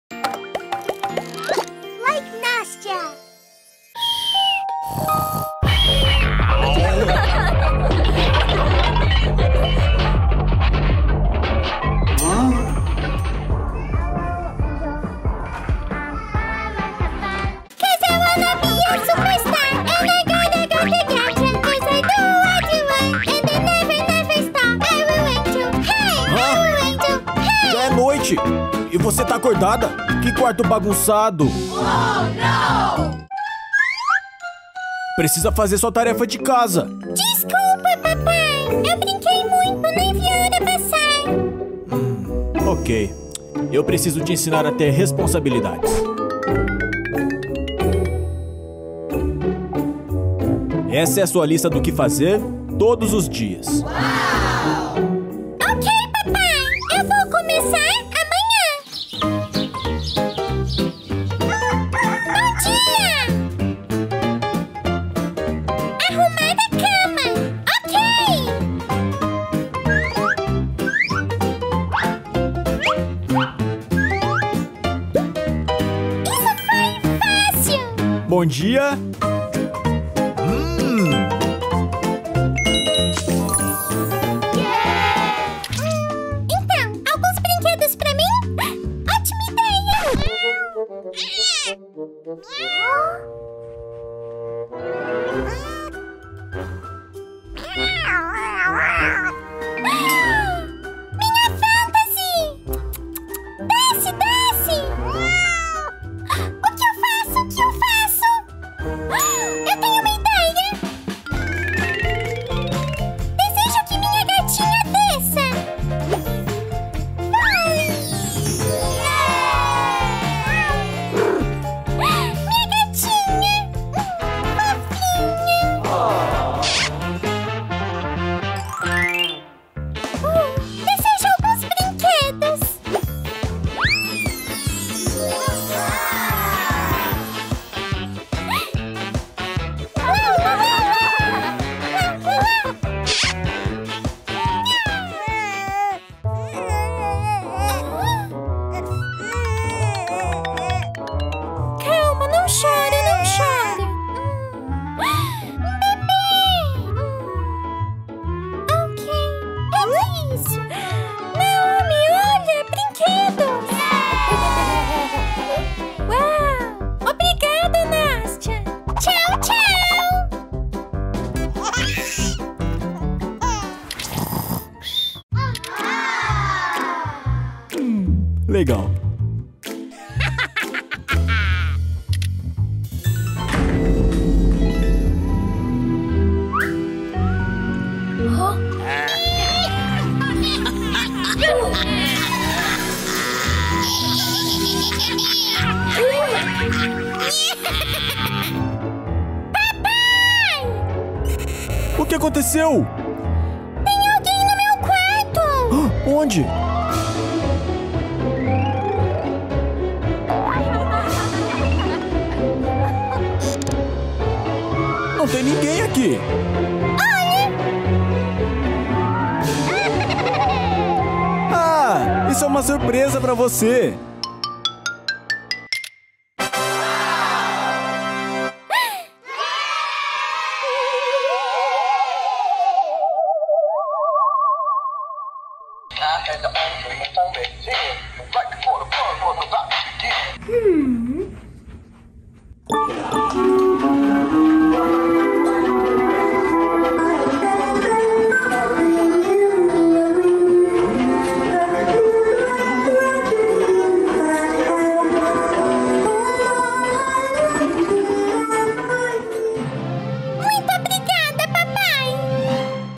Nastya aprende a fazer responsabilidades diárias, série de vídeos para crianças
Nastya aprende a cumprir as responsabilidades diárias com uma lista de tarefas! Nastya se torna uma garota responsável com a ajuda de uma lista de tarefas. Para cada tarefa útil que ela executa corretamente, ela recebe uma recompensa de seu pai na forma de uma estrela. Aprenda a fazer uma programação diária para organizar o tempo e as tarefas diárias. Série de vídeos para crianças.